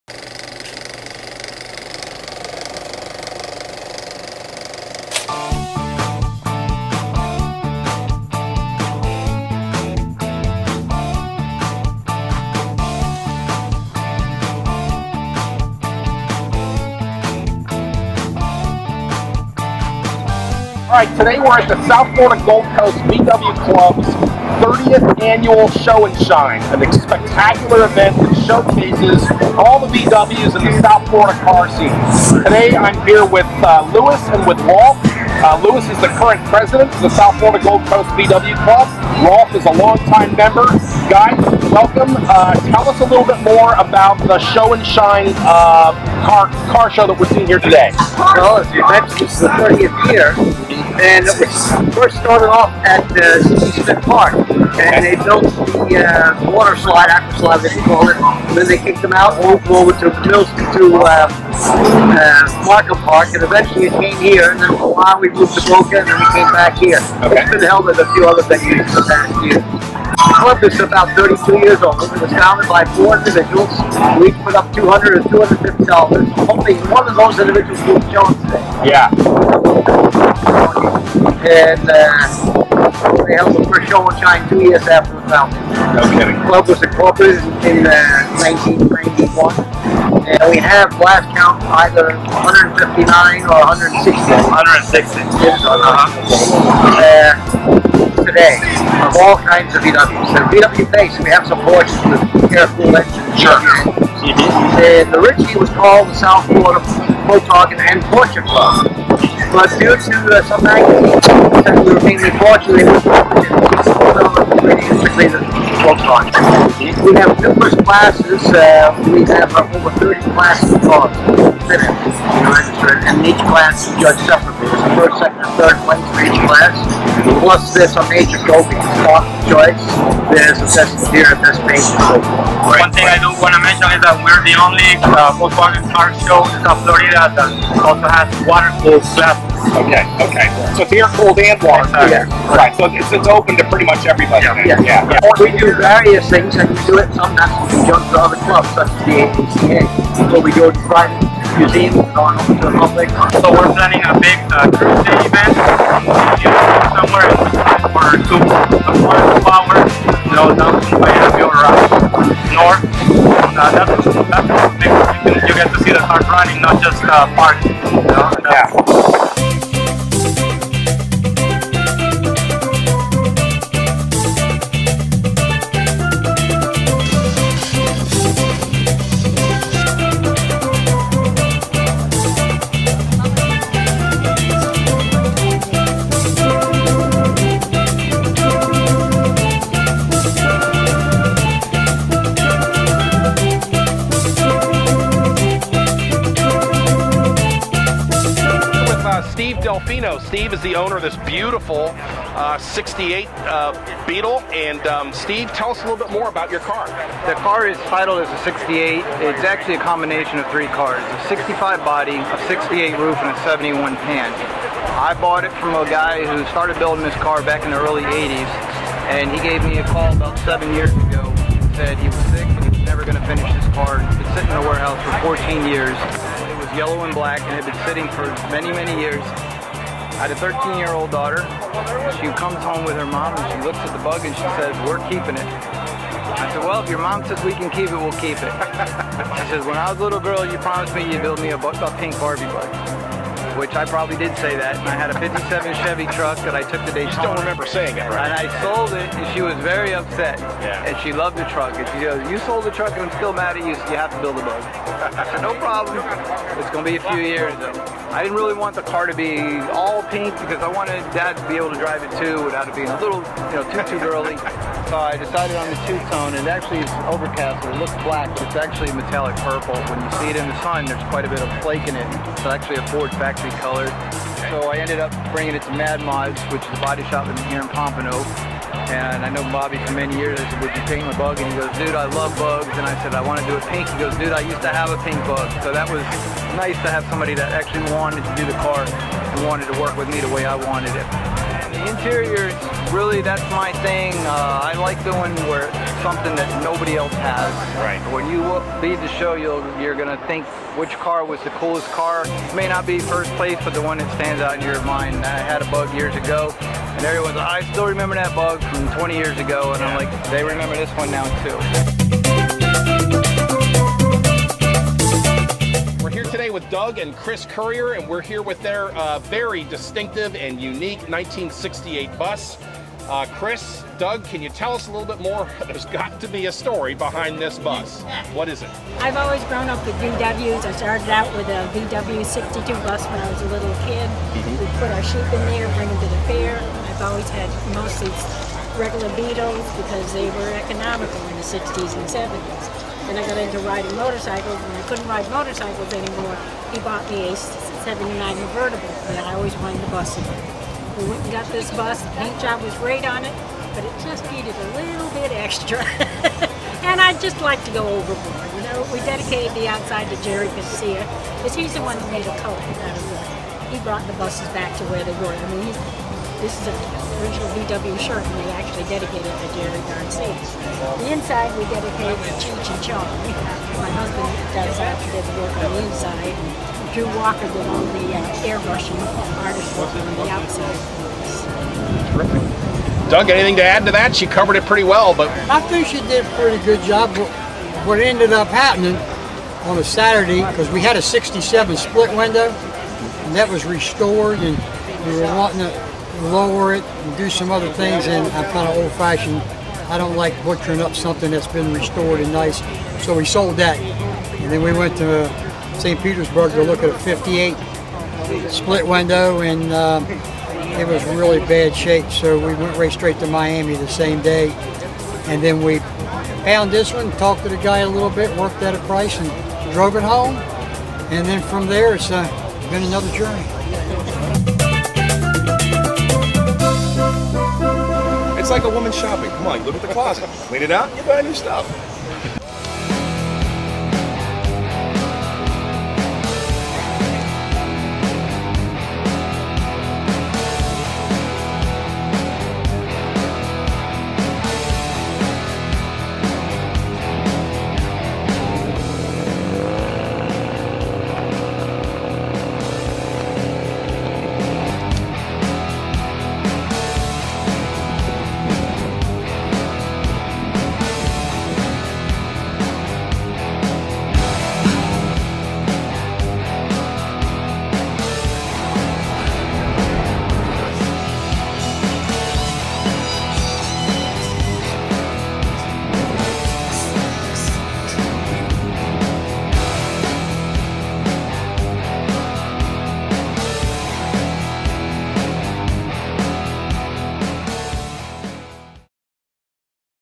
2013 VW Show And Shine Pompano Beach - Car Show TV
Car Show Television cover the VW Show And Shine in April 2013 that was presented by the South Florida Gold Coast VW Club. The event was designed to showcase all of the VWs engulfed in Florida's car scene.There were 160 VWs featured in the show and there are 30 classes that are judged separate- car owners are competing for first, second or third place in their particular classes. We came across a beautiful 1968 VW Beetle, owned by Steve Delfino, with a poppin' pink and metallic purple paint job.